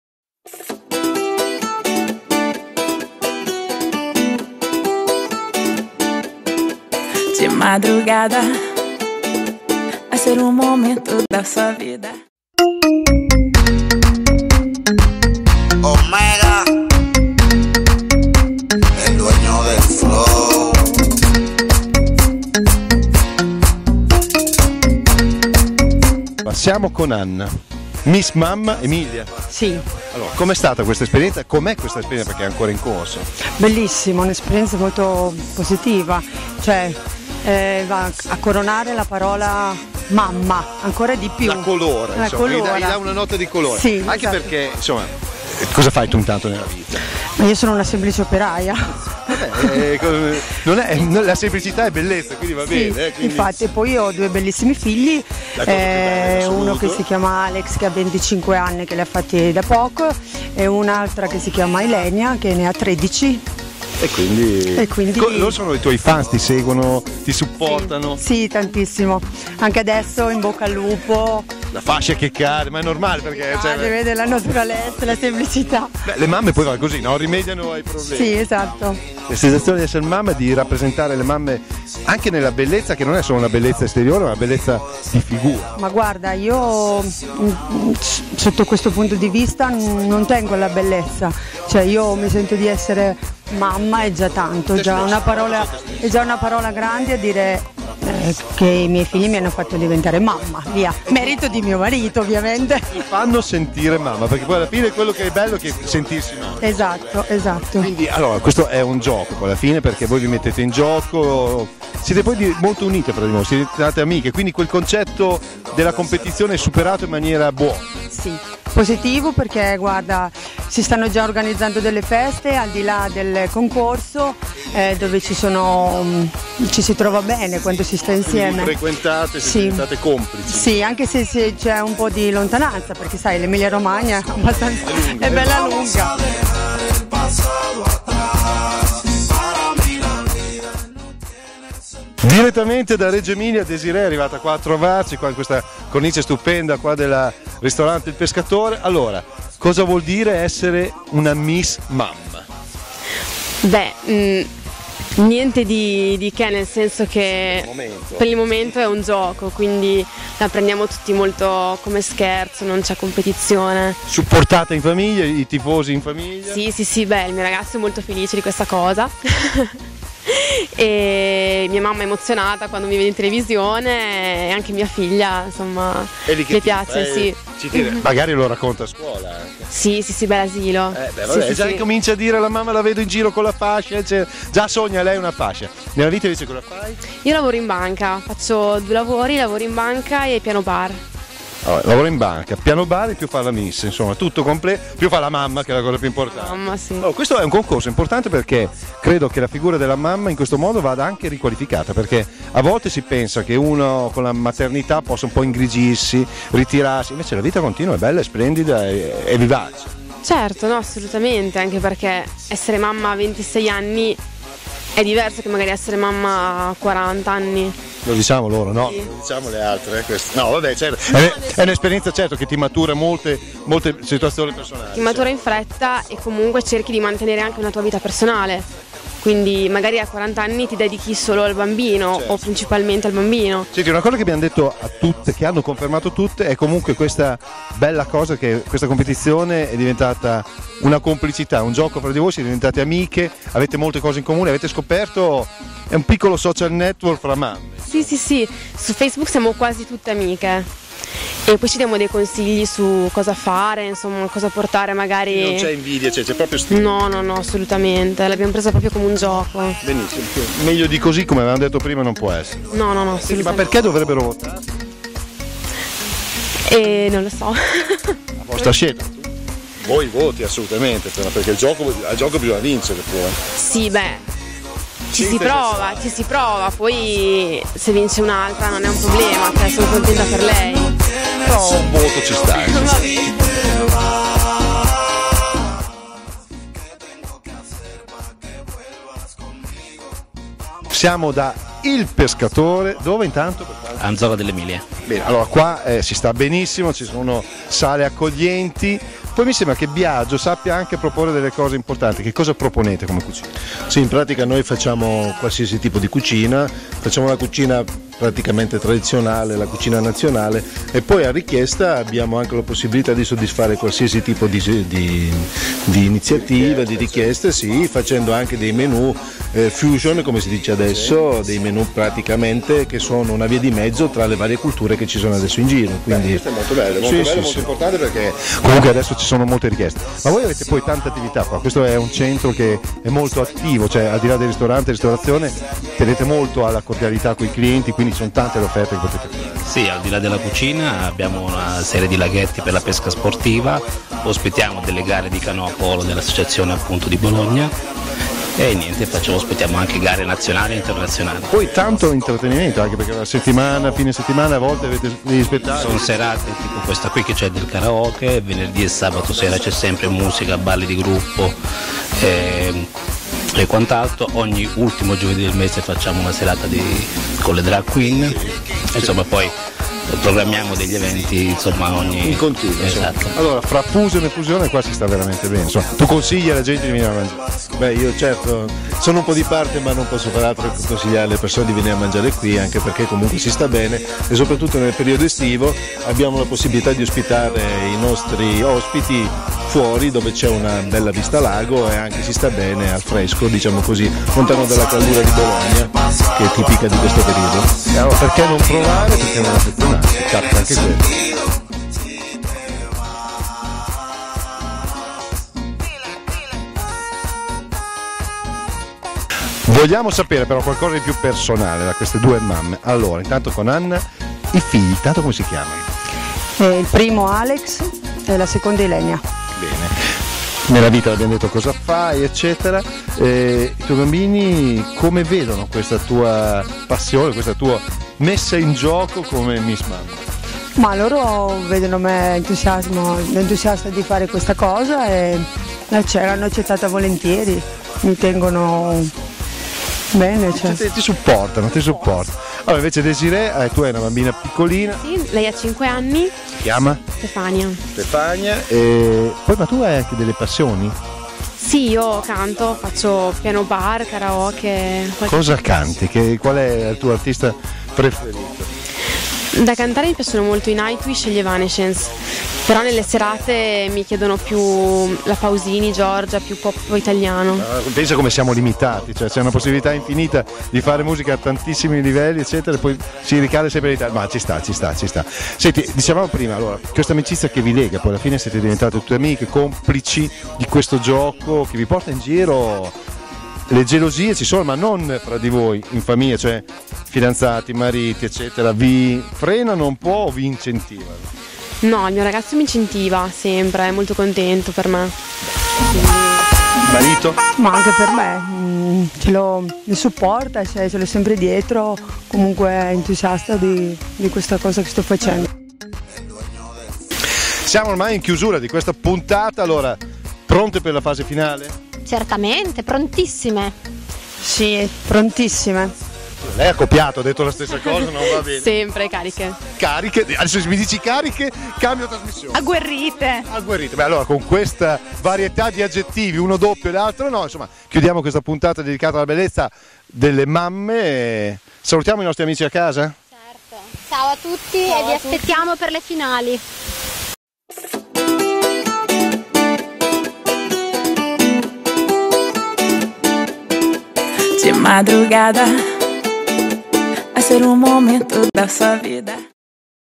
CASTIN GO 2^ puntata
a bellezza di una Mamma, come "cuore" di un Concorso, che vuole valorizzare il ruolo materno e femminile delle Donne. Te.ma. organizza tutto questo a Gatteo mare in Piazza della Libertà. Selezioni regionali curate dall'Agenzia Note di Moda di Massimiliano Padulazzi e dal Direttore Artistico Francesco Chiofalo.